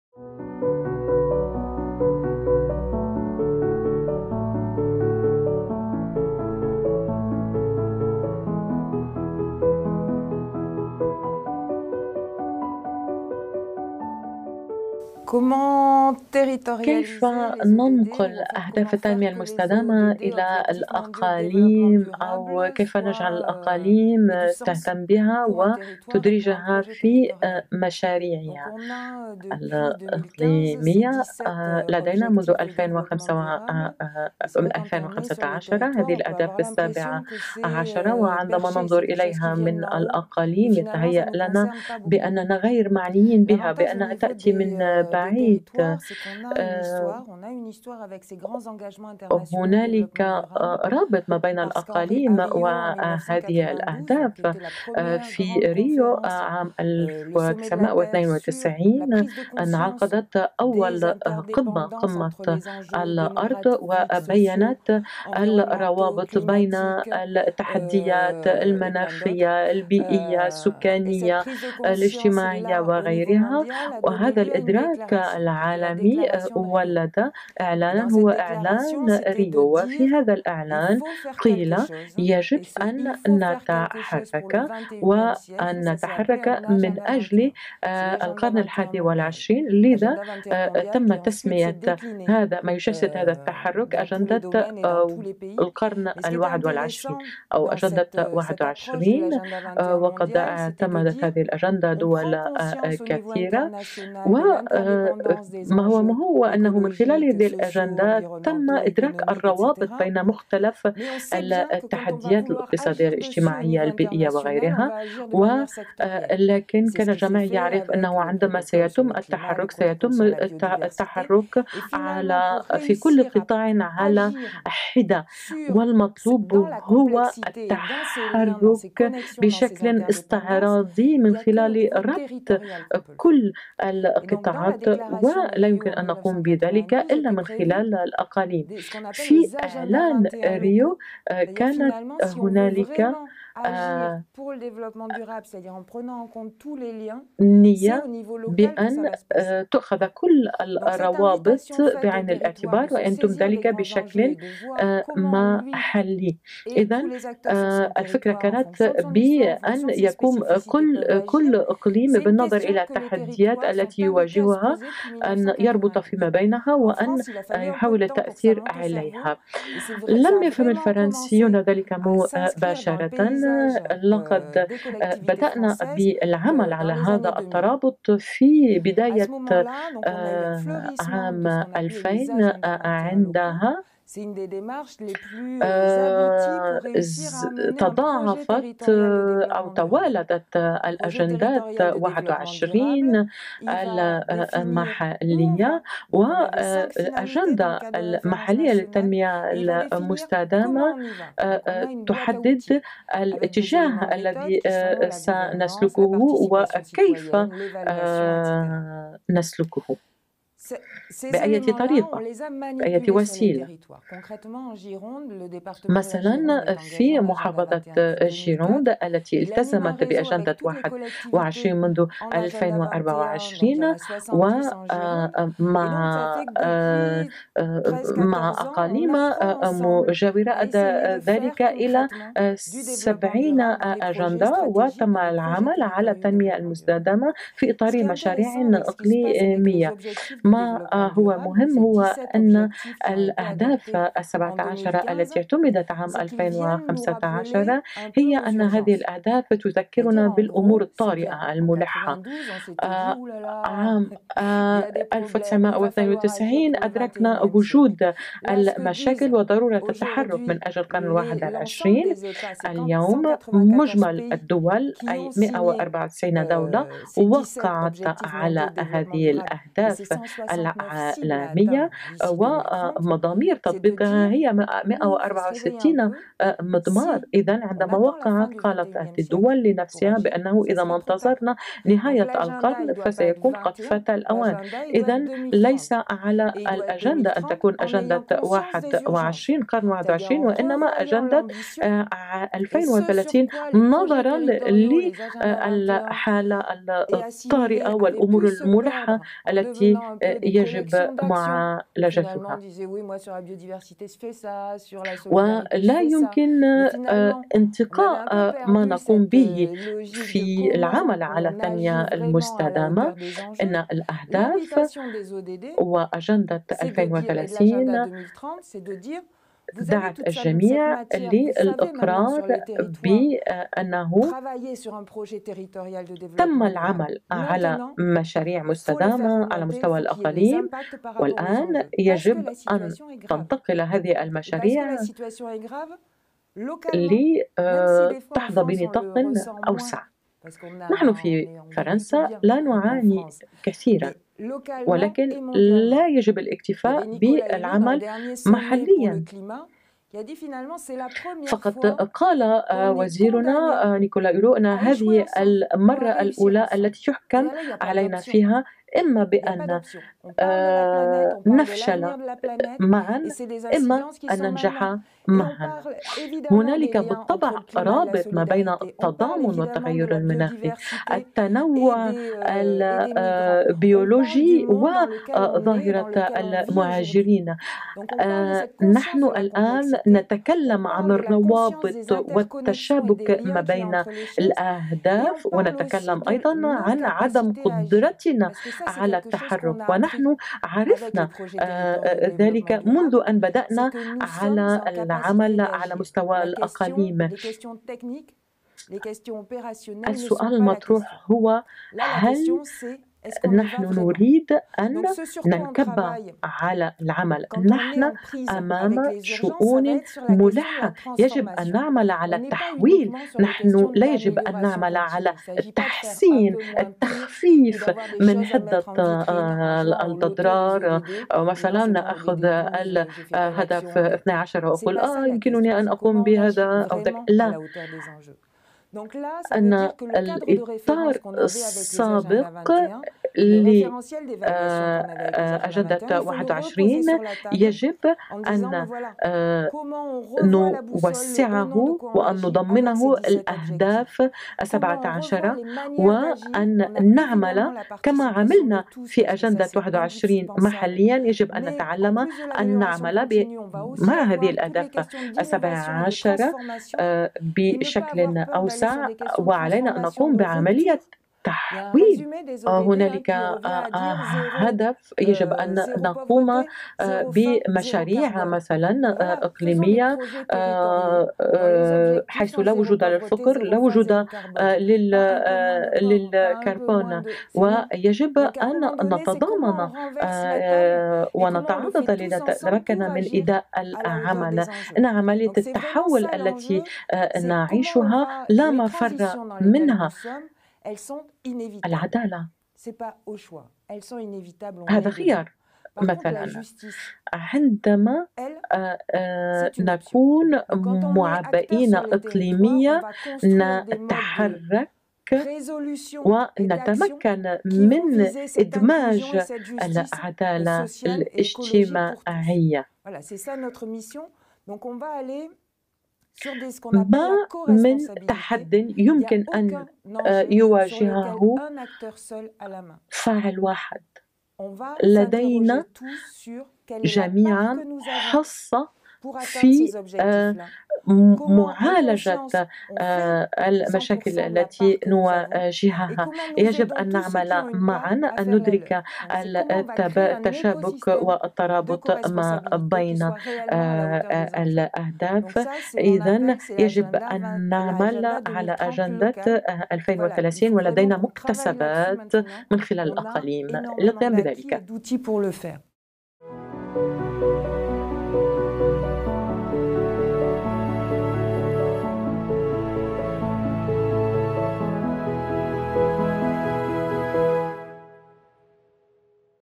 تنفيذ أهداف التنمية المستدامة في الأقاليم : نحو أجندة محلّبة ل2030
تناقش "جولي شابو"، المسؤولة عن مهمة جدول أعمال القرن 21 في دائرة جيروند (فرنسا)، في هذا الفيديو تملك السلطات المحلية لأهداف التنمية المستدامة: أولًا، تتّبع تاريخ هذه المسألة قبل تسليط الضوء على العناصر التي تبدو اليوم ضرورية لاستجابة السلطات للتحديات المعاصرة الكبرى بشكل أفضل.